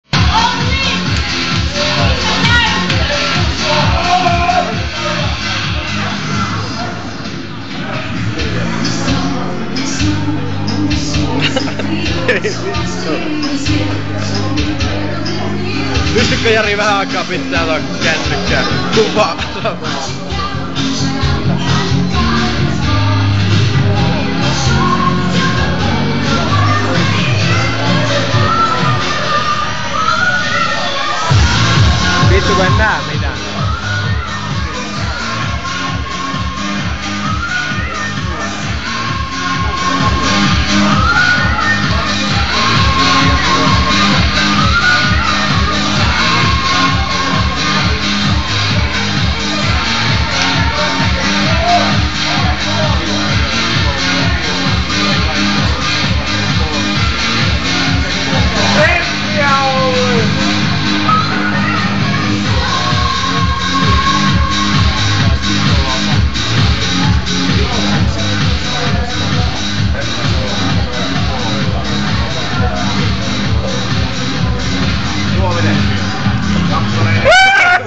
Drunken Dancer
Drunken dancer scares off other dancers on the dancefloor